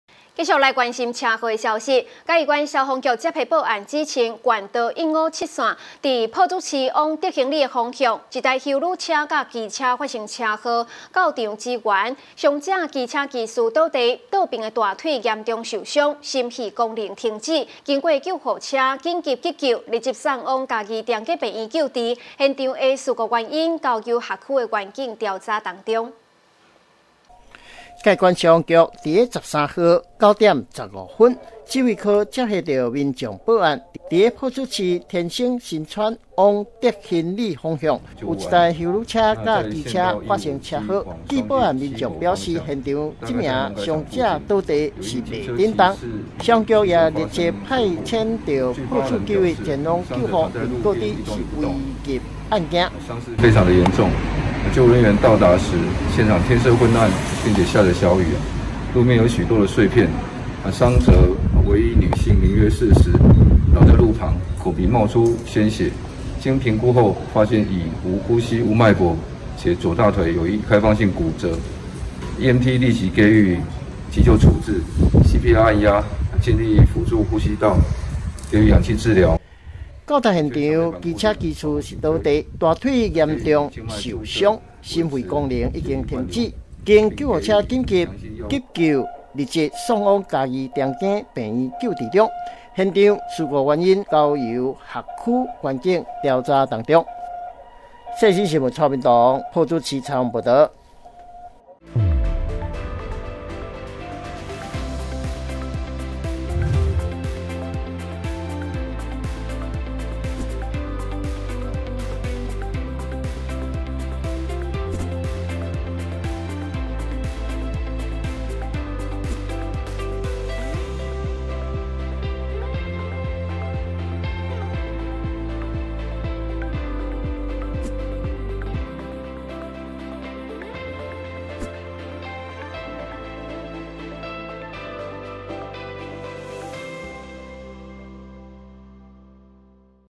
世新新聞 休旅車機車車禍肇事 女騎士心肺停止生命危急
111年02月14日 (一) 世新新聞

休旅車機車車禍肇事 女騎士心肺停止生命危急

嘉義縣消防局接獲報案，指縣道157於朴子市往德興里方向，一部休旅車與機車發生車禍，到場救援，傷者機車騎士倒地，左大腿嚴重受傷，心肺功能停止，經救護車緊急急救，立即送往嘉義長庚醫院救治中，現場事故原因交由轄區員警調查中。